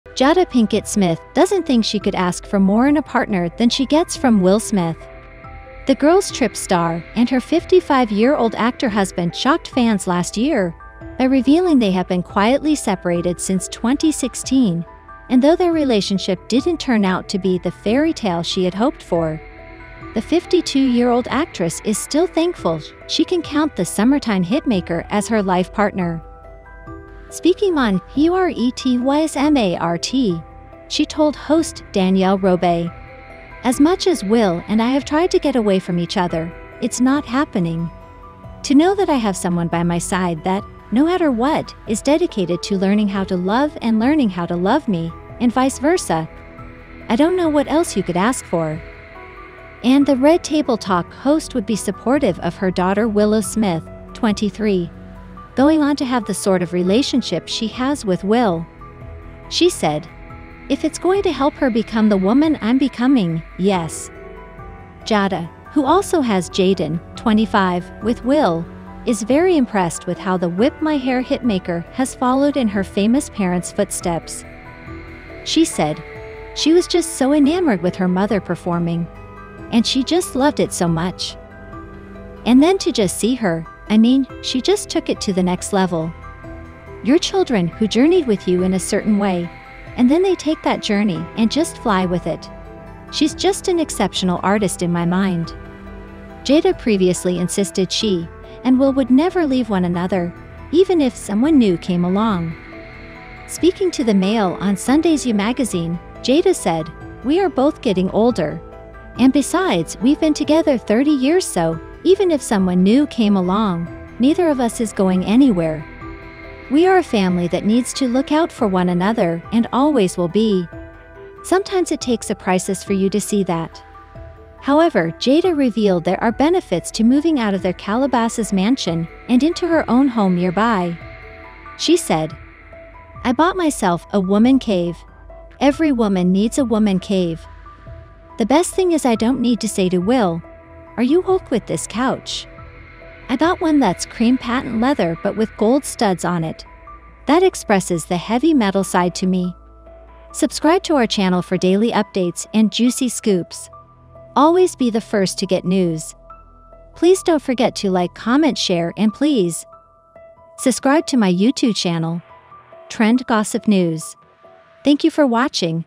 Jada Pinkett Smith couldn't ask for more than Will Smith's love and support || LatestNEWS
Jada Pinkett Smith couldn't ask for more than Will Smith's love and support

WELLCOME TO OUR YOUTUBE CHANNEL